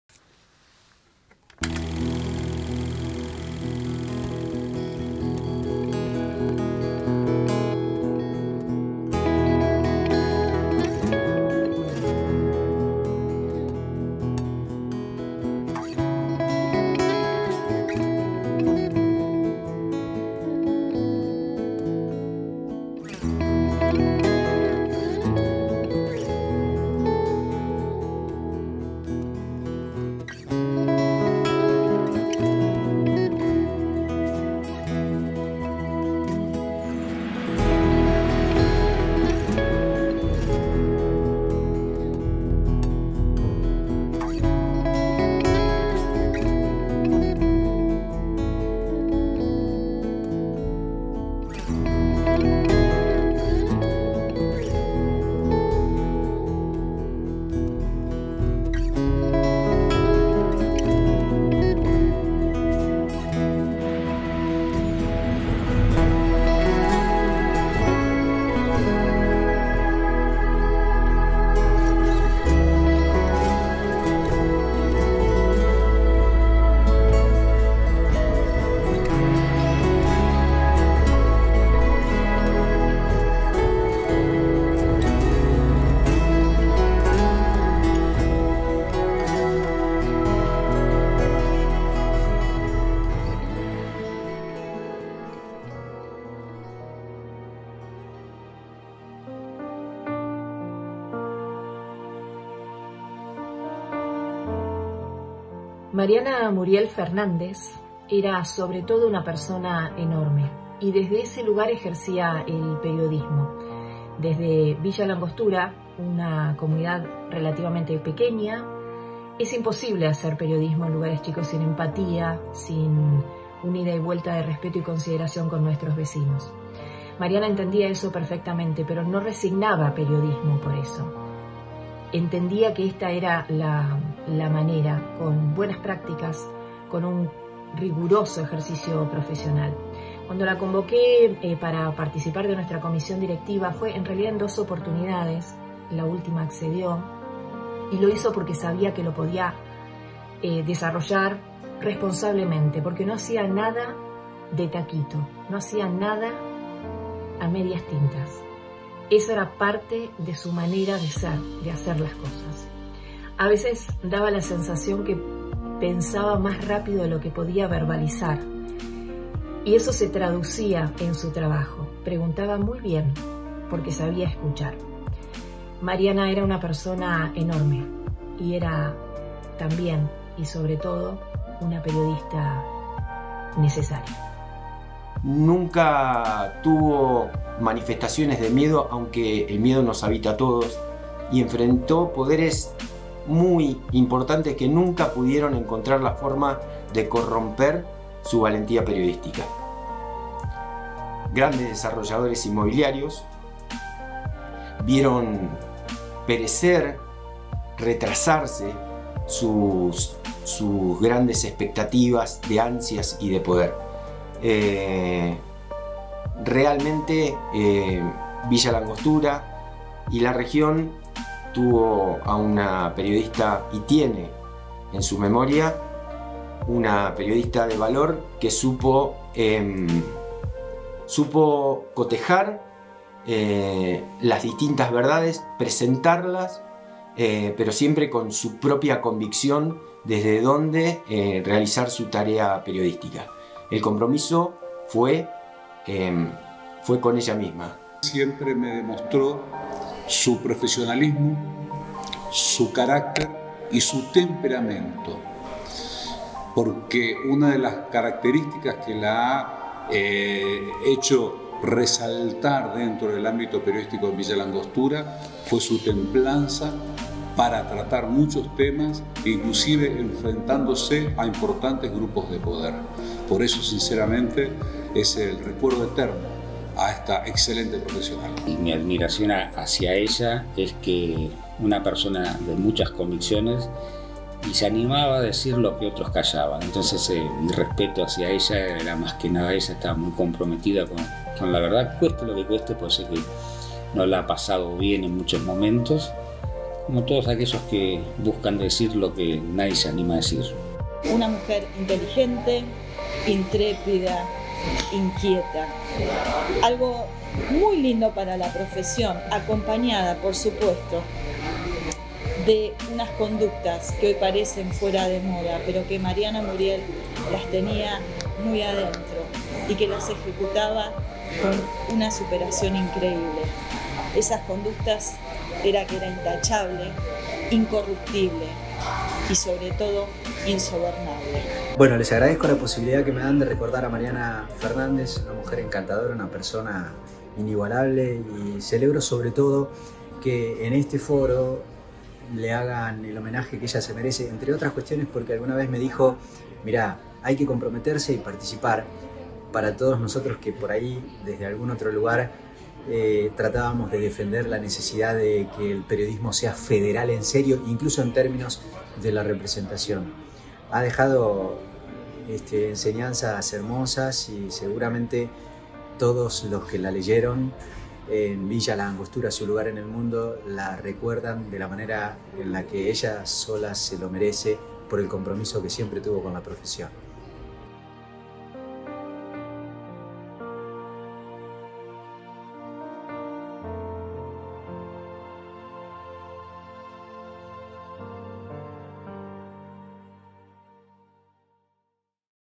Homenaje a Mariana Fernández
En el 11mo Congreso de Periodismo Digital homenajeamos a Mariana Muriel Fernández, socia de FOPEA de Neuquén, quien falleció en 2021.

Mariana fue también parte de nuestro Monitoreo de Libertad de Expresión e integró la comisión directiva de nuestra organización. .